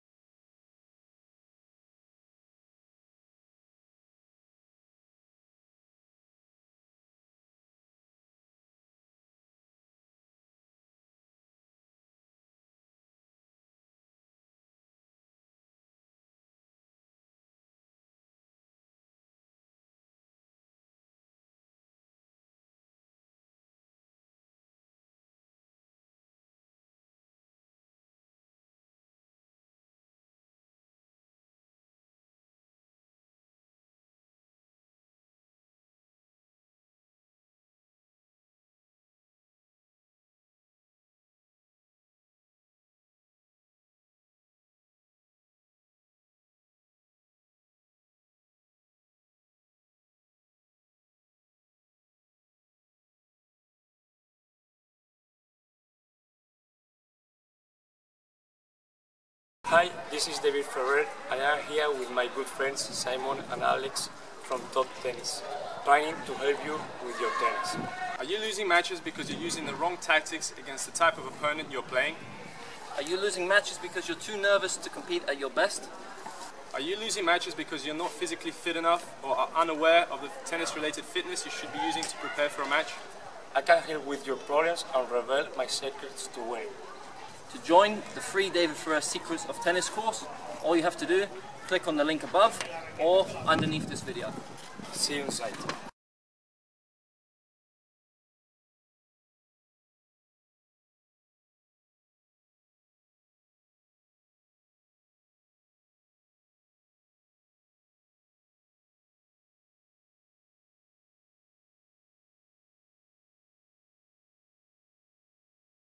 Federer Serve & Volley In Slow Motion | Poetry In Motion
Win more matches with David Ferrer as your tactical coach.

In this video we have the great Roger Federer's serve and volley in slow motion.
Poetry in motion - enough said.
Notice he follows the line of the ball when coming into net and the timing of his split step.

Upgrade your tennis game today, for FREE – click here for our free course